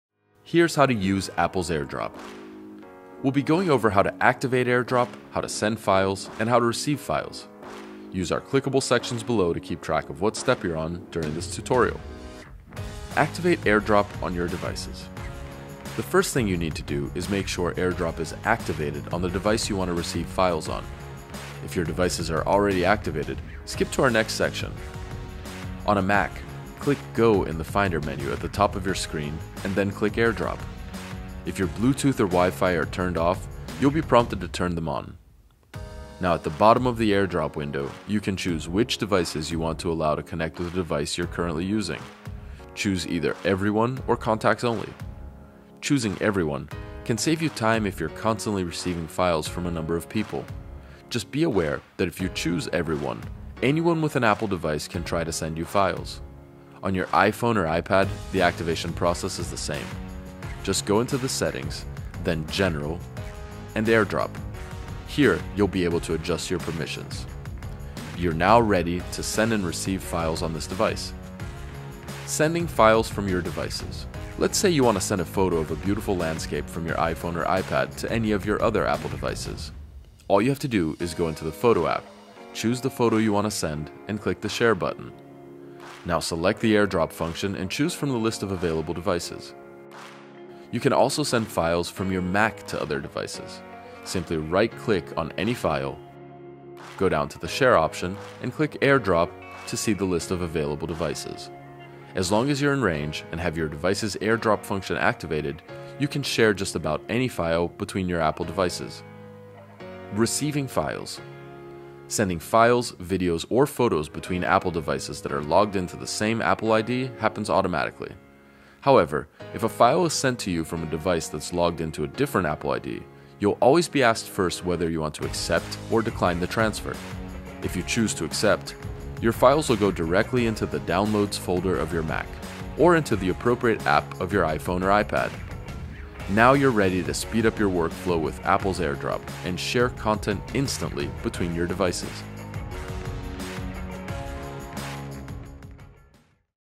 How To Use AirDrop
0:00 Intro
0:12 Activating Airdrop on Mac
0:56 Activating iPhone & Ipad
1:11 Sending Files
1:50 Receiving Files

AirDrop is a simple tool that allows you to share files between Apple devices. Here is how to send and receive files using AirDrop.